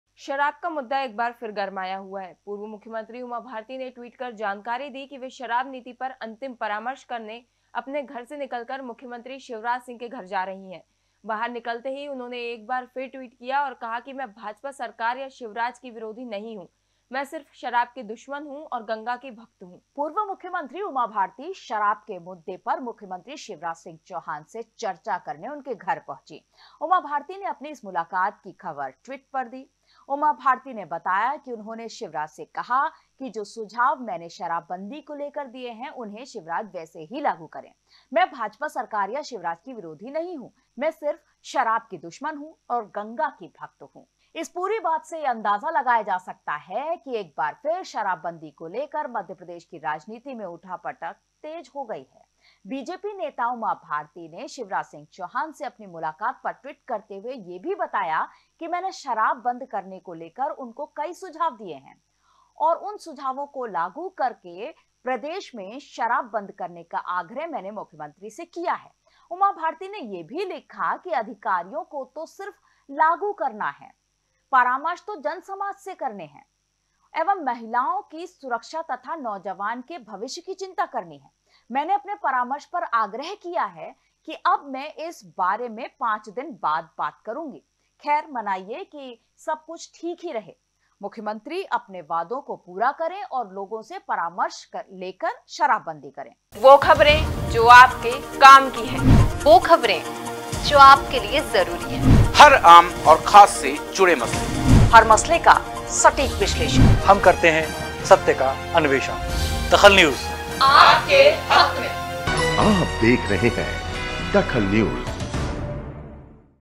DAKHAL NEWS शिवराज की नहीं शराब की दुश्मन हूँ-उमा भारती
शिवराज की नहीं शराब की दुश्मन हूँ-उमा भारती
गाँधी जयंती तक शराब बैन करे शिवराज-उमा भारती